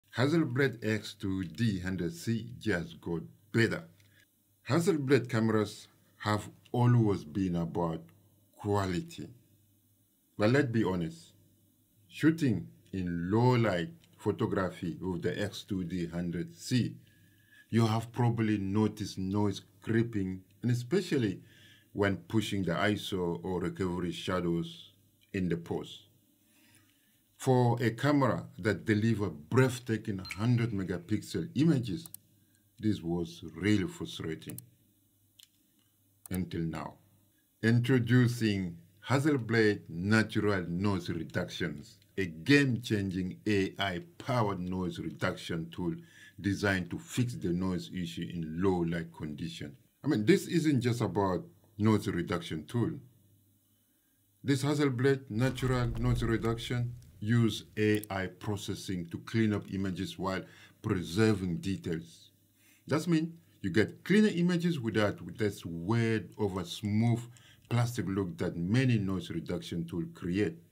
Hasselblad X2D 100C HNNR – AI Noise Reduction is a GAME CHANGER! 🔥
📸 Hasselblad just FIXED low-light noise! The X2D 100C now has HNNR (Hasselblad Natural Noise Reduction)—an AI-powered noise reduction tool that cleans up images without losing detail! But is it really a game changer for low-light photography? 🌙✨

🔥 Quick Breakdown:
✅ Before & After Test – See the difference!
✅ AI-Powered Noise Reduction – No more grainy night shots!
✅ Purity vs. Detail Mode – Which one is better?